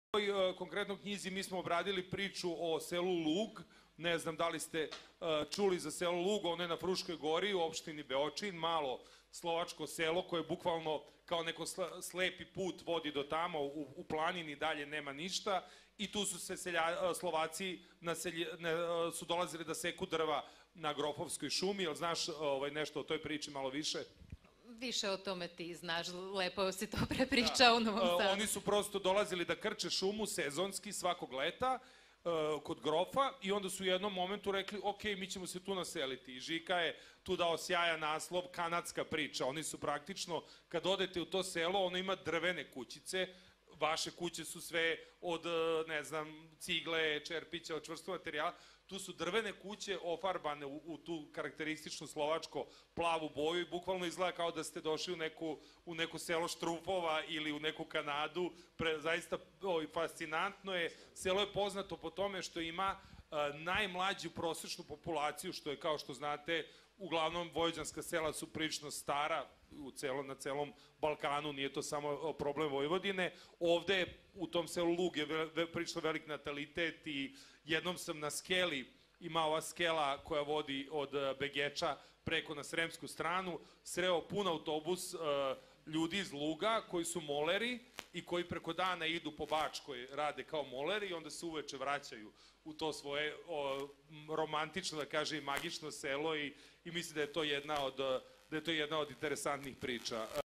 Robert Čoban: Selo štrumfova usred Vojvodine – kuće ofarbane u plavo
Promocija knjige "Izgubljeni u ravnici" u Magliću

Učesnici:

Žikica Milošević, autor
Srđan Simić, predsednik Opštine Bački Petrovac
Vladimira Dorčova-Valtnerova, gl. urednica časopisa "Hlas Ludu"
Uranija Kozmidis Luburić, potomak Grka iz Bulkesa (roditelji - Ida Sabo i Filo Kozmidis)
Vasilios Proveleggios, Građanska stranka Grka Srbije
Christian Reissmüller, šef odeljenja za kulturu i štampu Ambasade Nemačke
Vinka Marjanović, matičarka iz Maglića

Moderator: Robert Čoban, izdavač